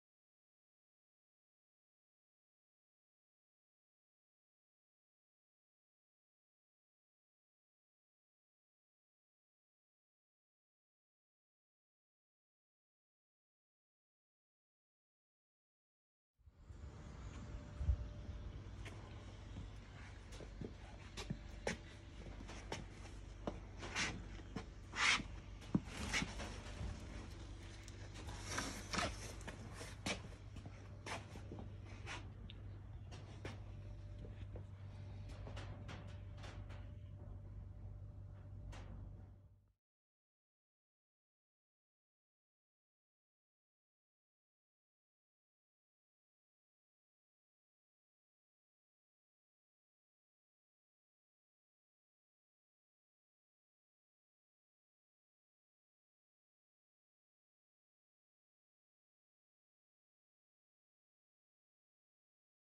CAMERA OBSCURA Storage Shed Revealed
A reveal as to how the storage shed packing box camera obscura was made.